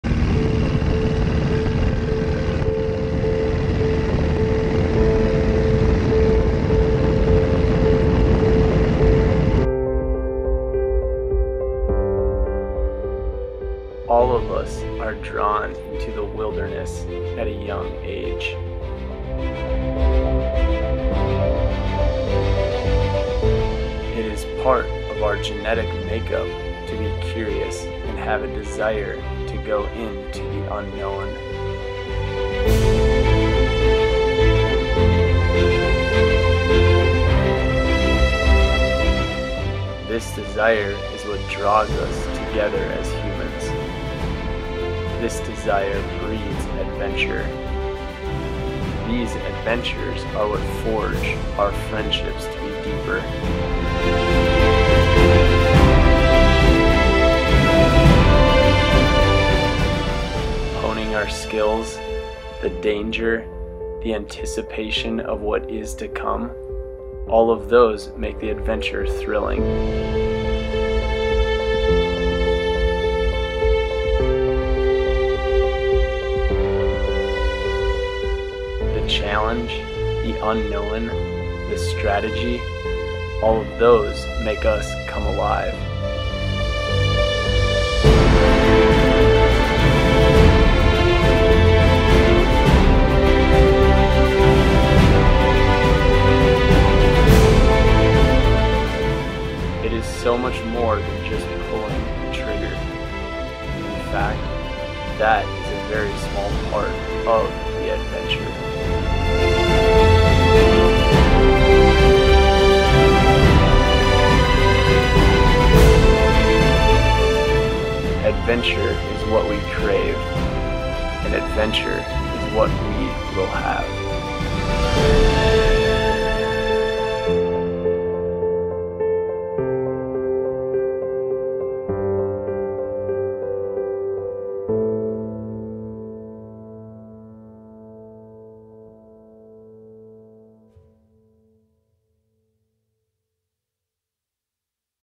The Reason: DIY Alaska Outdoors
The reason we go in to the wilderness to explore is simple. We are made to do it!

I come alive on DIY Adventures, but especially ones that take place in the wilderness of Alaska!